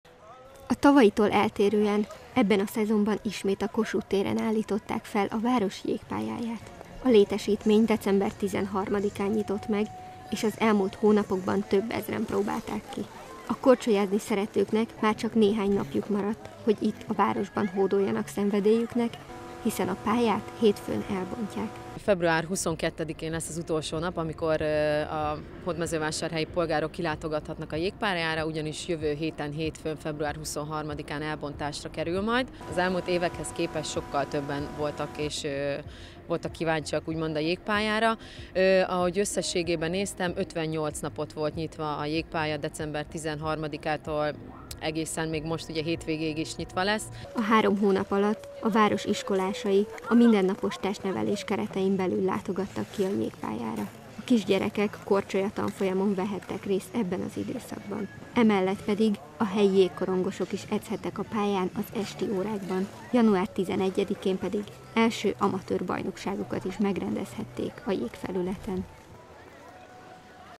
Vasárnap bezár a jégpálya
A hét végéig, azaz február 22-ig használhatják még a Vásárhelyiek a jégpályát. A december közepén üzembe helyezett létesítményt az elmúlt hónapokban több ezren használták. Ez idő alatt a kisgyerekeknek korcsolya tanfolyamot is szerveztek. A jégkorong szerelmesei pedig esténként használhatták a pályát.